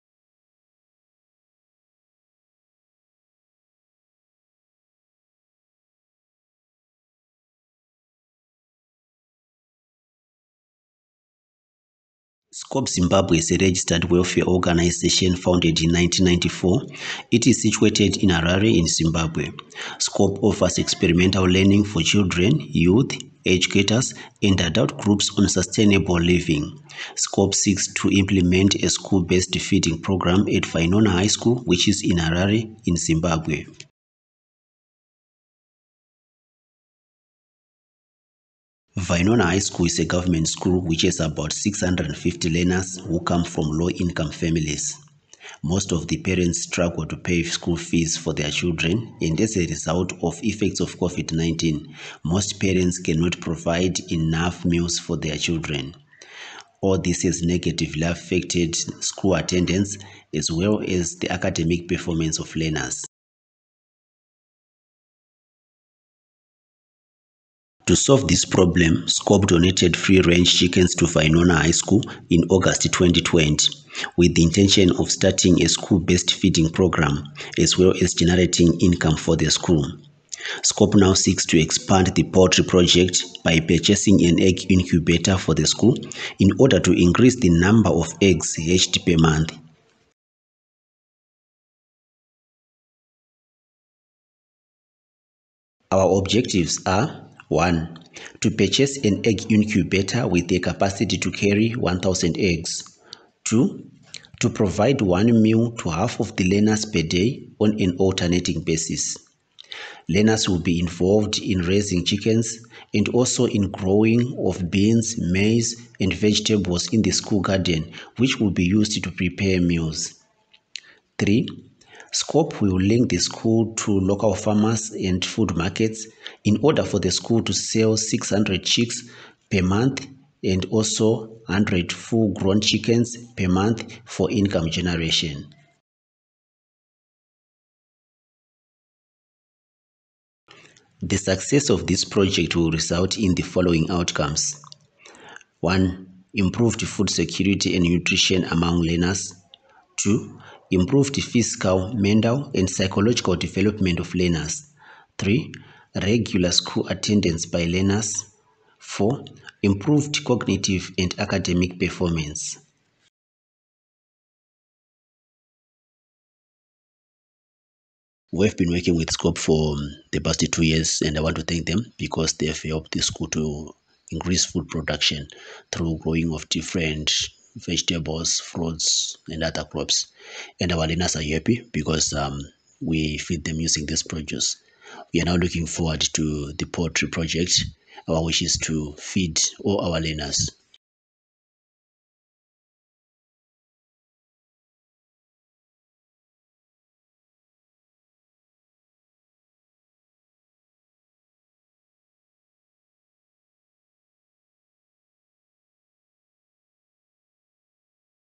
Make it Grow: School Agro-Business for Student Meals
A participatory video proposal, made by members of SCOPE, Zimbabwe.

This community proposal was created within the frame of the University of Sheffield's Institute for Sustainable Food project, "Make it Grow: Supporting community based food initiatives to access grants through participatory video proposals".